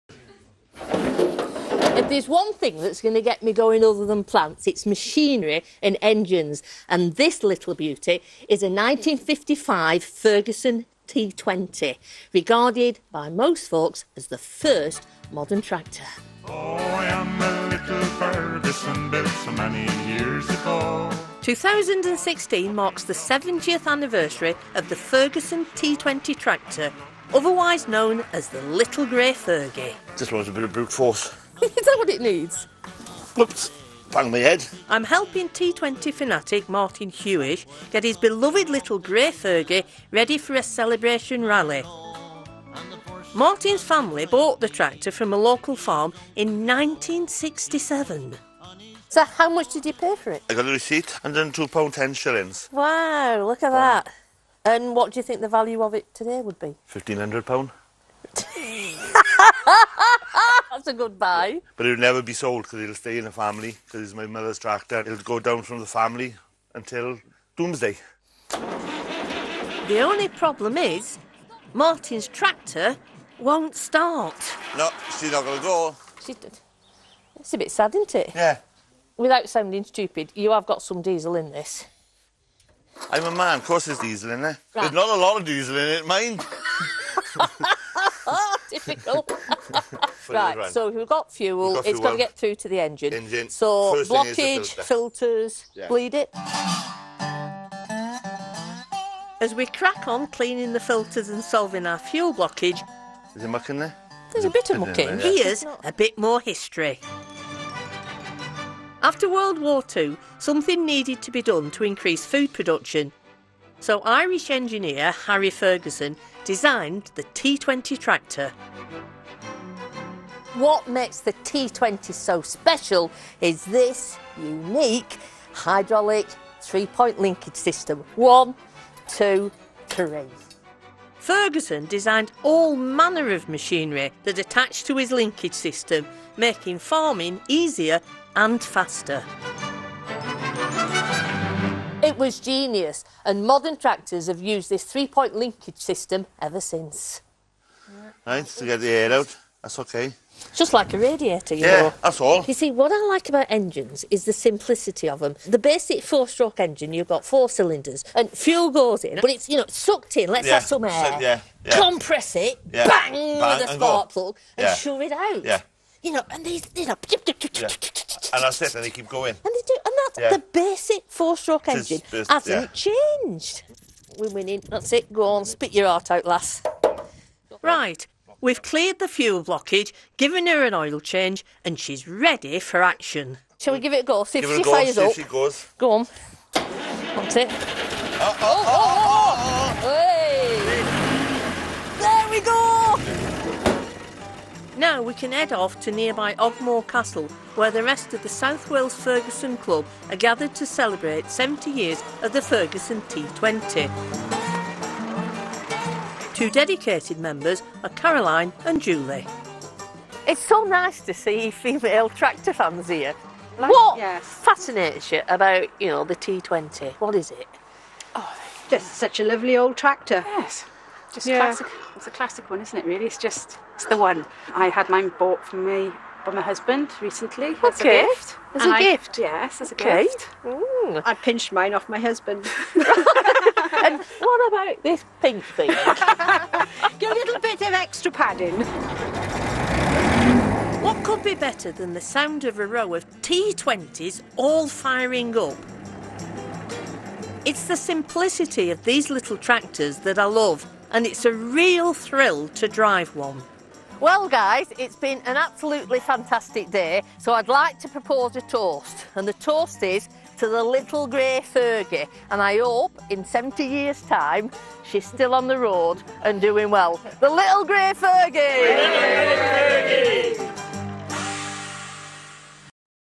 The Little Grey Fergie tractor (UK) - BBC - 2nd May 2016
Regraded by many as the first modern tractor, hear the story of the Little Grey Fergie and its three point linkage system, that made it so versatile, that the system is still used today.